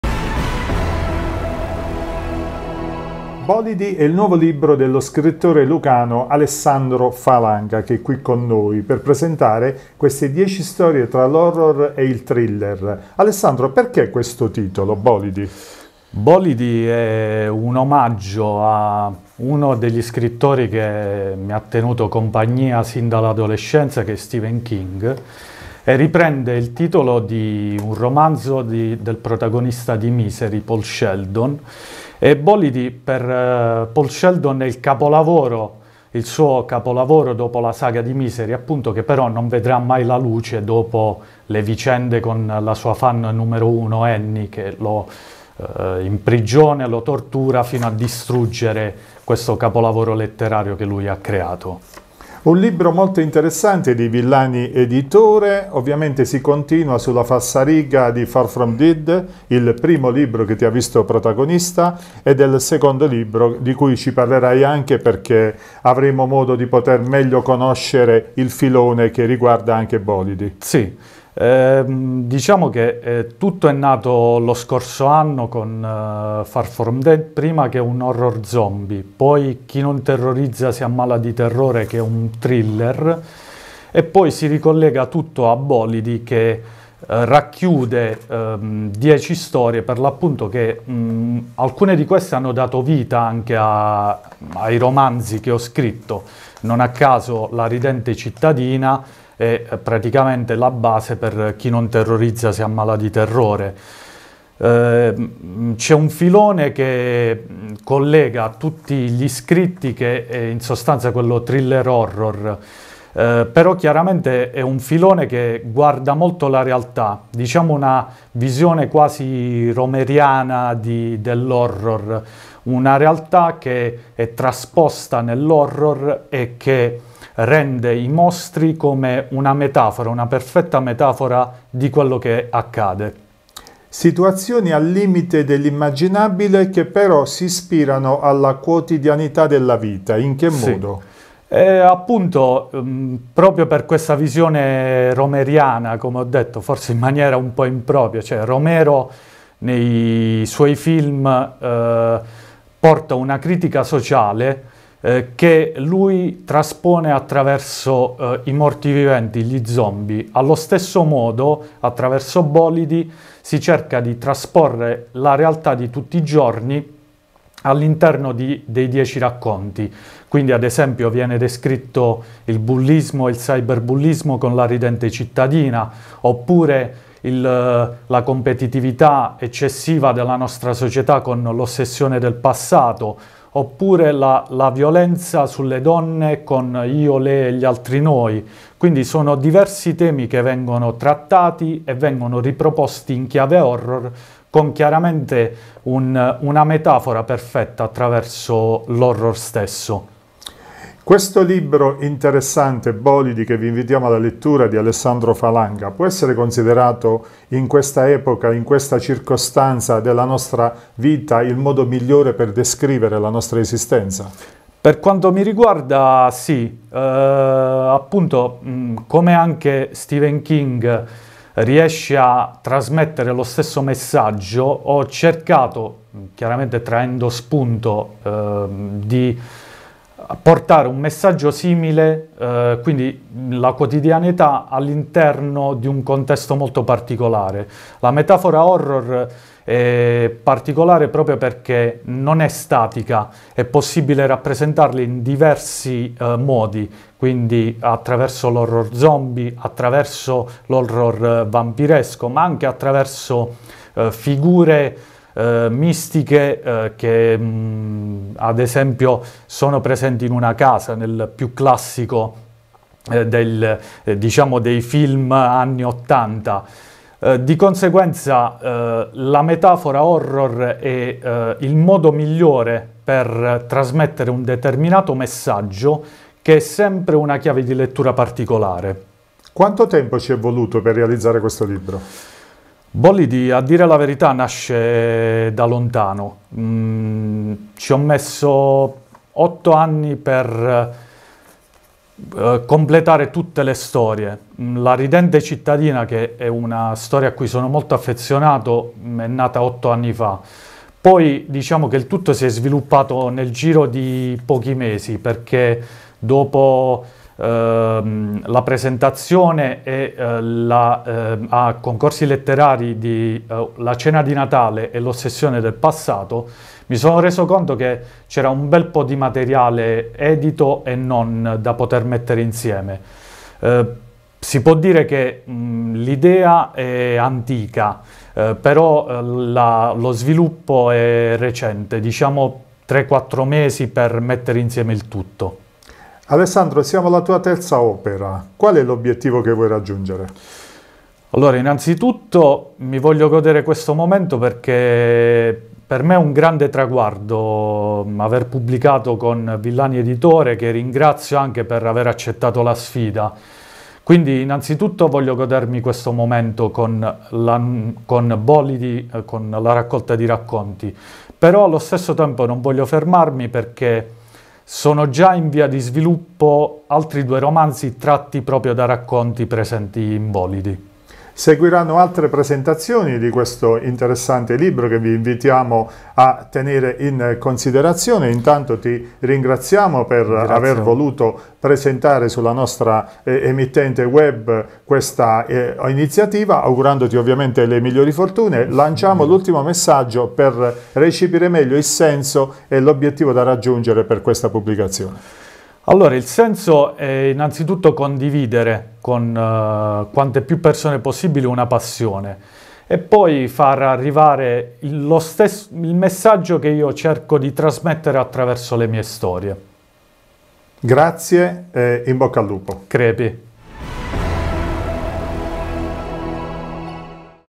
TG7 Basilicata. Presentazione del libro BOLIDI di Alessandro Falanga
TG7 Basilicata 21 Ottobre 2021.

BOLIDI" DI ALESSANDRO FALANGA, RACCOLTA DI RACCONTI TRA HORROR E THRILLER
Altra pubblicazione importante per il 36enne scrittore e giornalista Alessandro Falanga di Barile che già da alcuni anni si sta facendo conoscere ed apprezzare positivamente in svariati ambiti professionali tra cui appunto quello della letteratura. Negli ultimi tempi il giovane scrittore e giornalista di Barile, laureato con il massimo dei voti in Scienze Politiche presso l'Università di Salerno nel 2014 e in Scienze Politiche delle Relazioni Internazionali presso la Alma Mater Studiorum di Bologna nel 2019, ha pubblicato un suo nuovo ed interessante volume questa volta ispirato però al genere horror-thriller dal titolo "Bolidi". Un libro, edito dalla famosa casa editrice lucana "Villani Editore" con sede legale a Calvello, davvero interessante che racchiude al suo interno dieci storie tra il genere horror e il thriller in cui si narra di fantascientifici ed immaginari zombie, vampiri, mostri ed anche di situazioni al limite dell'immaginazione che si ispirano alla quotidianità della vita e a figure che circondano l'esistenza di tutti gli esseri umani come marito, moglie, figli, genitori e amici. Secondo l'autore il genere horror rappresenta il modo migliore per descrivere la nostra esistenza e, sempre a suo parere, l'horror può essere considerato una sorta di metafora perfetta per la sua capacita' di trasporre un determinato aspetto della vita in un momento particolare imponendo una riflessione più ampia sul mondo che ci circonda. Un libro interessante e tutto da scoprire che, grazie anche alla professionalità della casa editrice Villani Editori, è disponibile ed acquistabile in tutte le librerie e gli store online Mondadori, Feltrinelli, Ibis e Libraccio. Un libro, "Bolidi", che avrà sicuramente grande visibilità e che saprà sicuramente deliziare i tanti appassionati del genere horror-thriller e non solo.
Francesco Preziuso